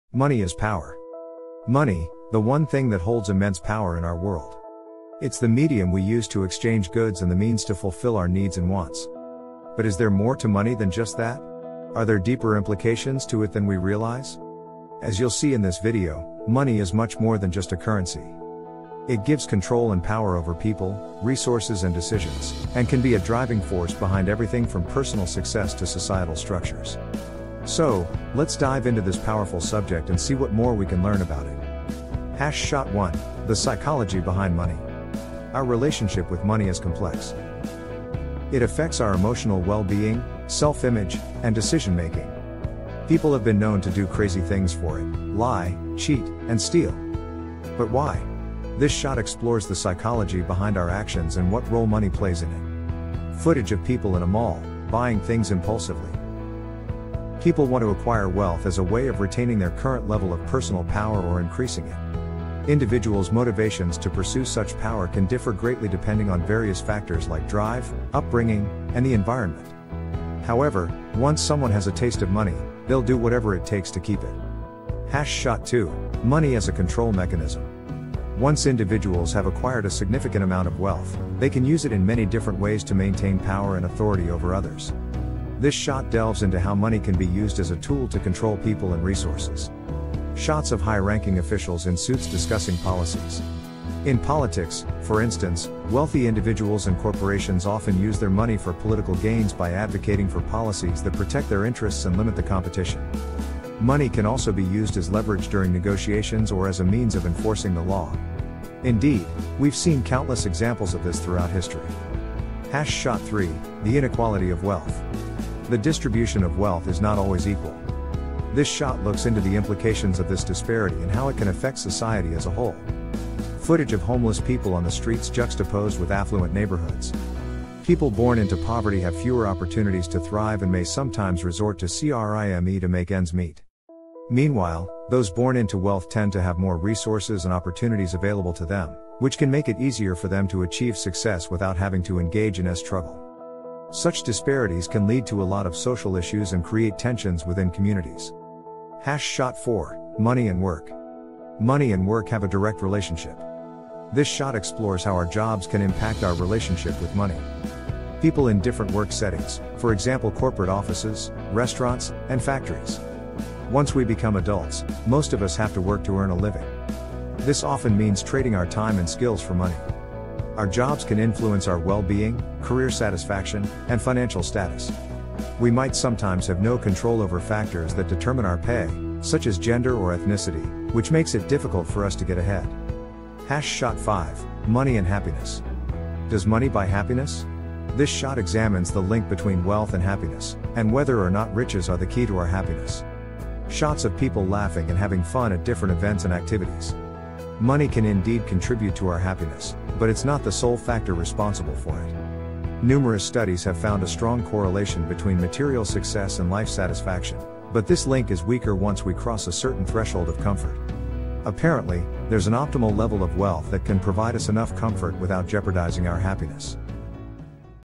The Psychology of Money: Why We Do Crazy Things to Get It | Money and Power
🧠💰 Delve into the intriguing realm of human behavior and finances with our latest video on "The Psychology of Money." 💸 Uncover the intricate ways our minds navigate the world of wealth, desires, and power.

🤯 Have you ever wondered why we make seemingly irrational choices when it comes to money? From impulsive spending to chasing risky investments, our actions often defy logic. Join us on this thought-provoking journey as we explore the hidden psychological forces driving these behaviors.

🔍 Dive deep into the fascinating connection between money and power. How does the pursuit of wealth impact our self-worth and relationships? Can an understanding of psychology help us make smarter financial decisions and achieve true fulfillment?

🔑 Unveil the secrets behind the allure of status, the fear of missing out, and the satisfaction of saving for the future. Renowned experts in psychology and finance lend their insights, shedding light on the mechanisms that govern our financial choices.

💡 Whether you're a seasoned investor, a curious mind, or someone striving for a healthier relationship with money, this video offers eye-opening revelations and actionable insights.

📺 Don't miss this captivating exploration of the interplay between the human mind, money, and power.

Looking to understand how money can affect your life, society, and the future? Join us in this video as we explore the power dynamics of money. From the psychology and control mechanisms of those who have it, to the inequality it creates and even its effects on our happiness. Watch, learn, and discover what you might not have known about money.